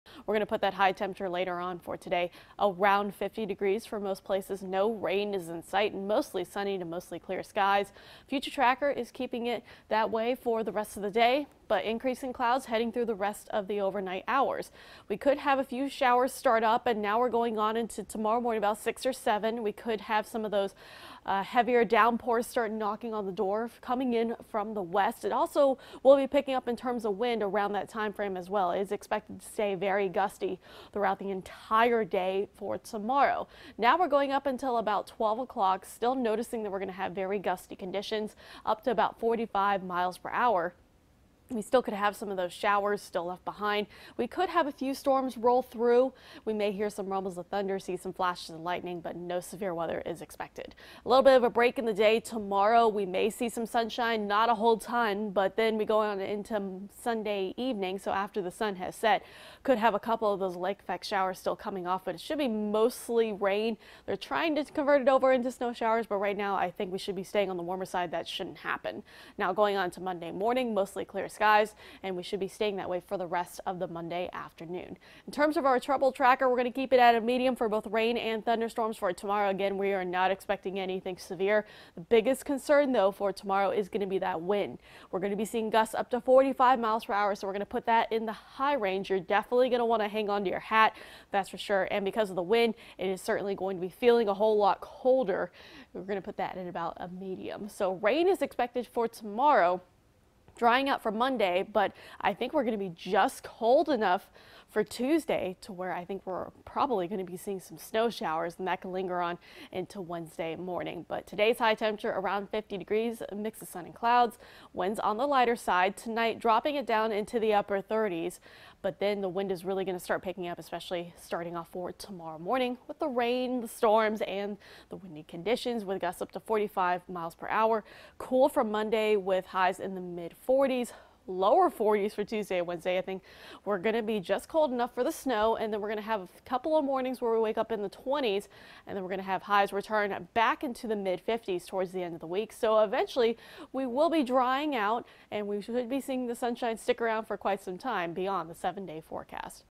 Sunny Saturday, rainy and windy Sunday
Sunshine and cool weather for Saturday. Showers and storms move through Sunday bringing along windy conditions.